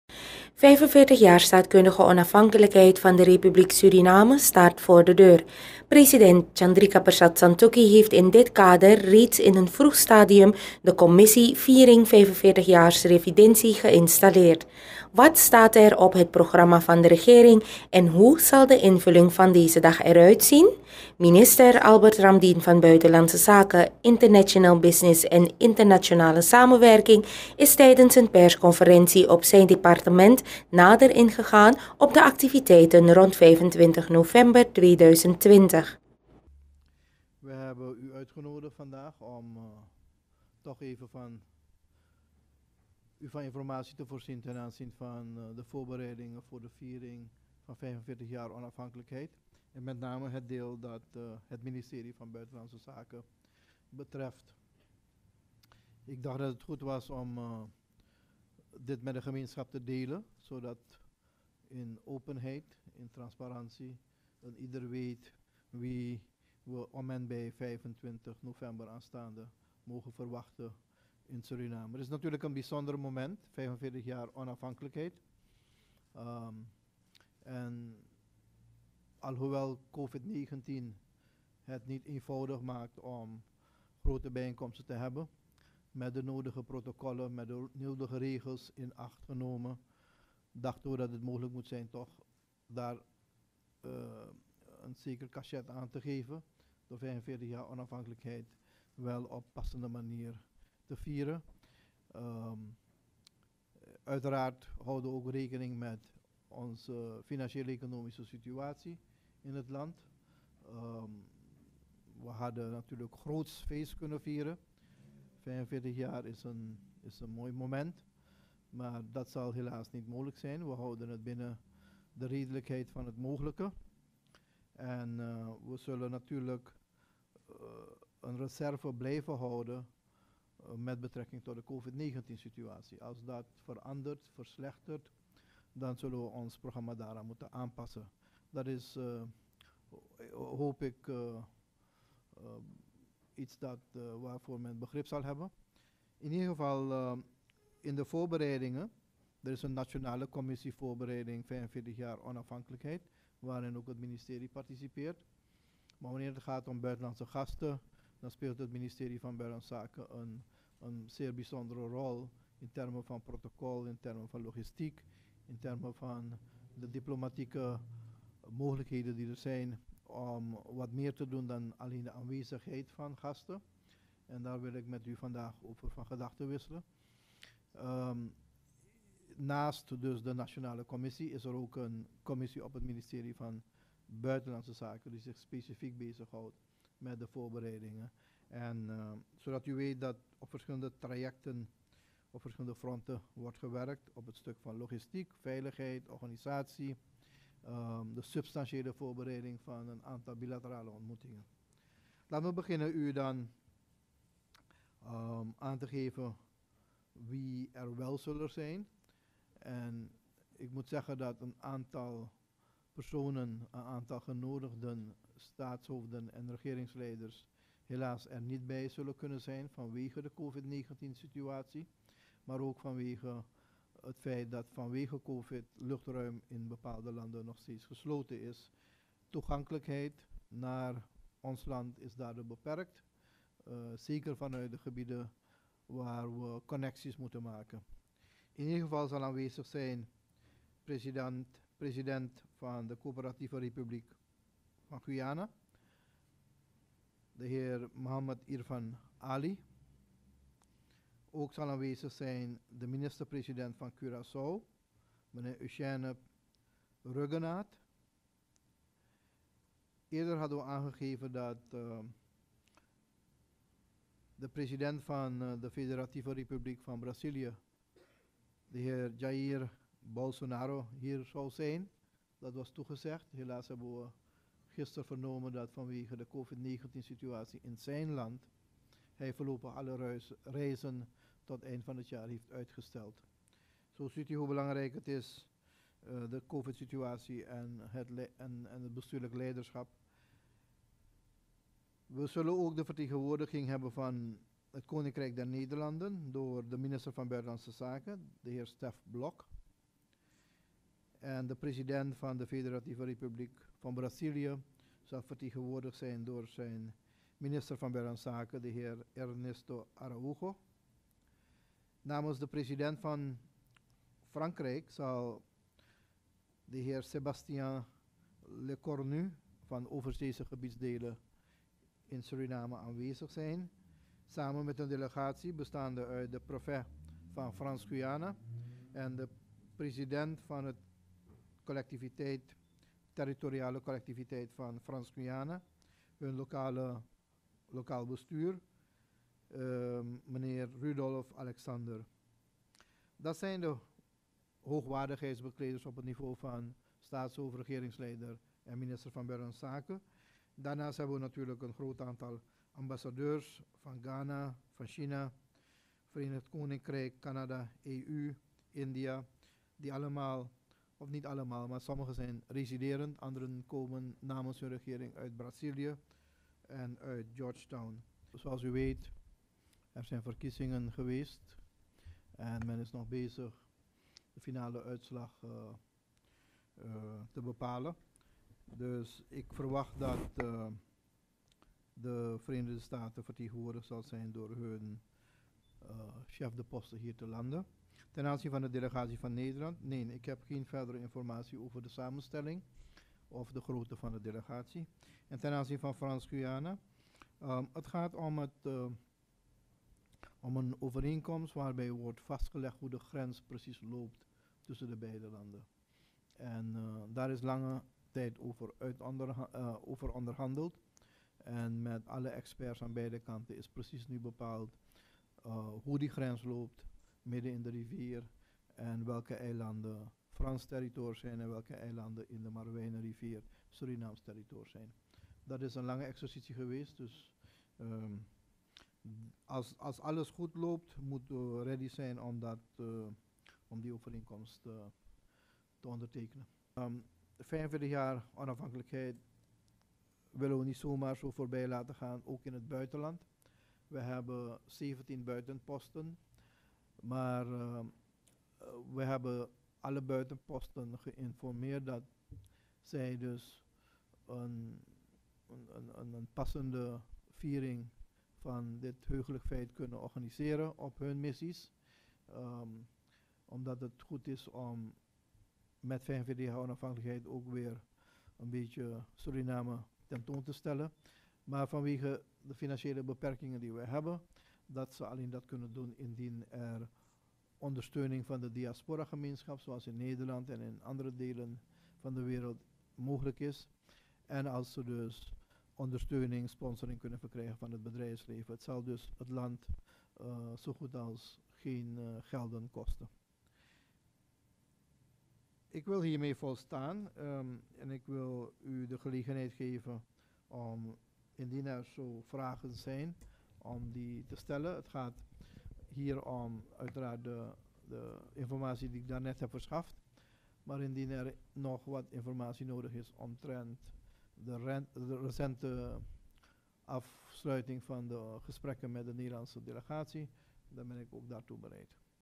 PERSCONFERENTIE BIBIS OVER 45 JAAR SREFIDENSI
45 jaar Staatkundige Onafhankelijkheid van de Republiek Suriname staat voor de deur. President Chandrikapersad Santokhi heeft in dit kader reeds in een vroeg stadium de Commissie Viering 45 jaar Srefidensi geïnstalleerd. Wat staat er op het programma van de regering en hoe zal de invulling van deze dag eruitzien? Minister Albert Ramdin van Buitenlandse Zaken, International Business en Internationale Samenwerking is tijdens een persconferentie op zijn departement nader ingegaan op de activiteiten rond 25 november 2020.